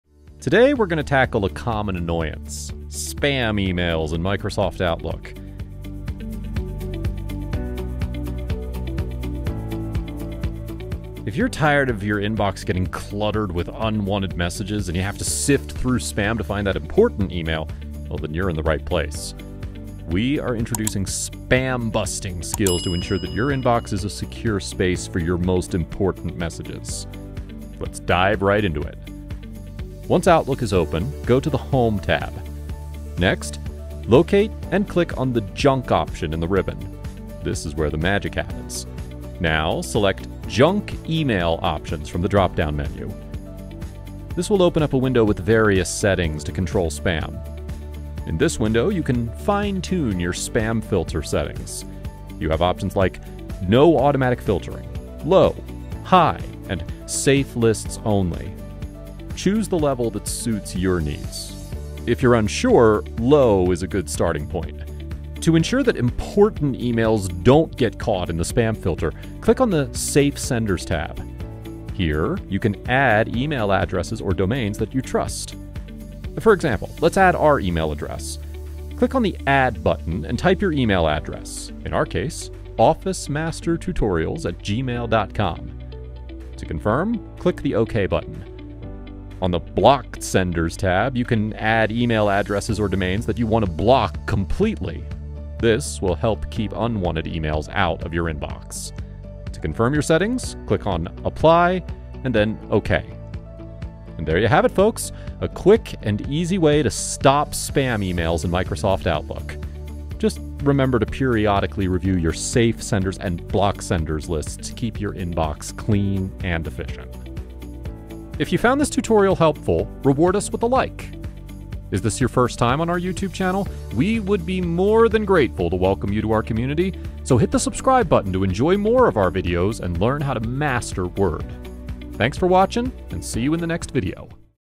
How to Stop Spam Emails in Outlook
Today, we're going to tackle a common annoyance: spam emails in Microsoft Outlook. If you're tired of your inbox getting cluttered with unwanted messages and you have to sift through spam to find that important email, you're in the right place. We are introducing spam-busting skills to ensure your inbox is a secure space for your most important messages. Let's dive right into it!